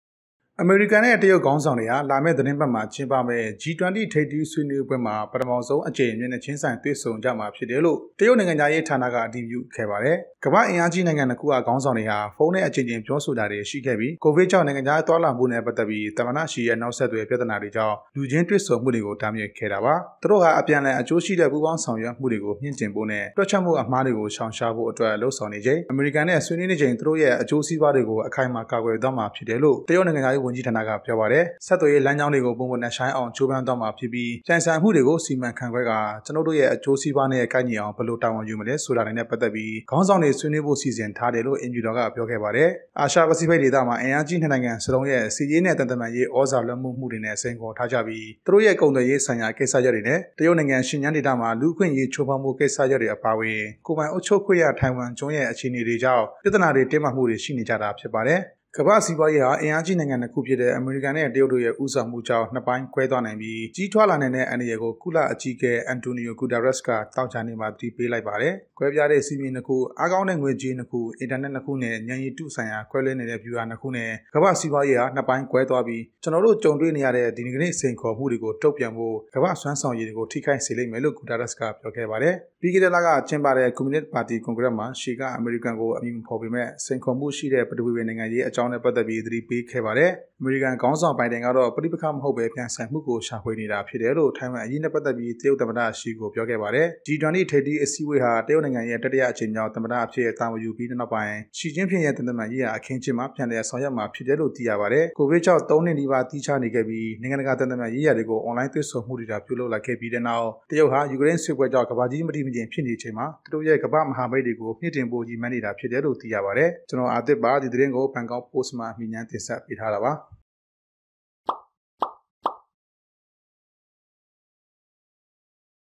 TLT Tv||- တရုတ်နဲ့ အမေရိကန်ခေါင်းဆောင်တွေ G-20 ထိပ်သီးအစည်းအဝေးမှာ တွေ့မယ်
အမေရိကန်နဲ့ တရုတ်ခေါင်းဆောင်တွေ ဟာ လာမယ့်သီတင်းပတ်မှာ ကျင်းပမယ့် G-20 ထပ်သီး ဆွေးနွေးပွဲမှာပထမဆုံးအကြိမ်မျက်နှာချင်းဆိုင် တွေ့ဆုံကြမှာဖြစ်တယ်လို့ တရုတ်နိုင်ငံခြားရေး ဌာနက အတည်ပြုခဲ့ပါတယ်။